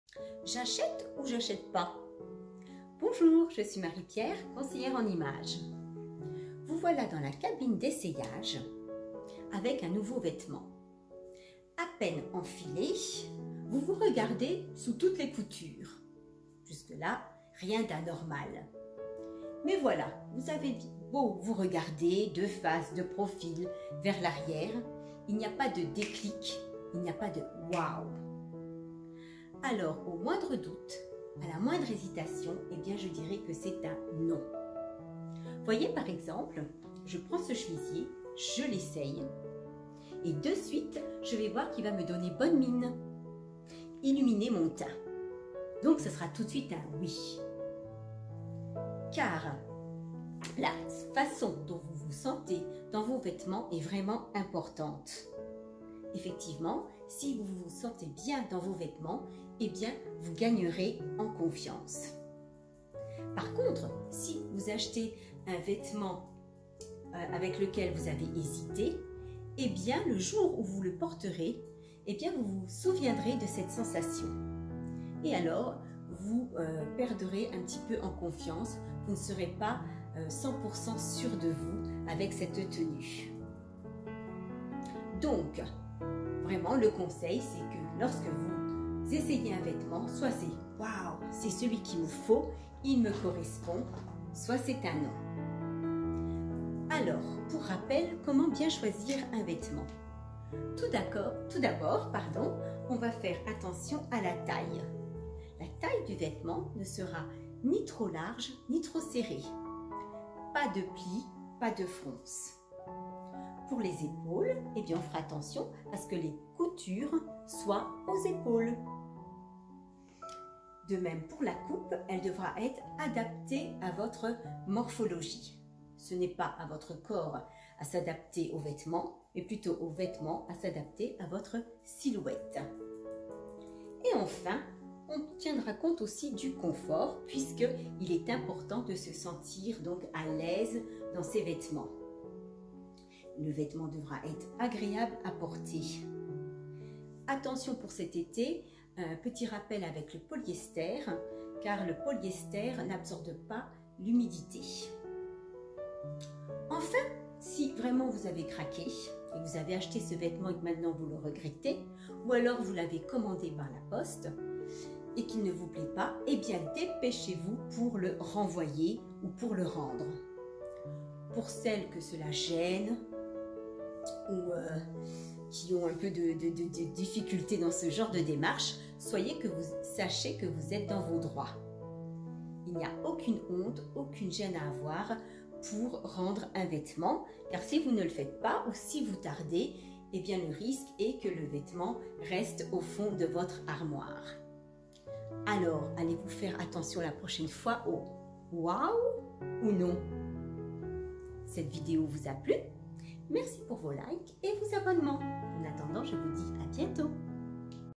J'achète ou pas ?
J'achète ou pas ce nouveau vêtement ?
00:00 Introduction
00:13 1ère impression
01:37 Bien choisir son vêtement
02:40 Retour de vêtement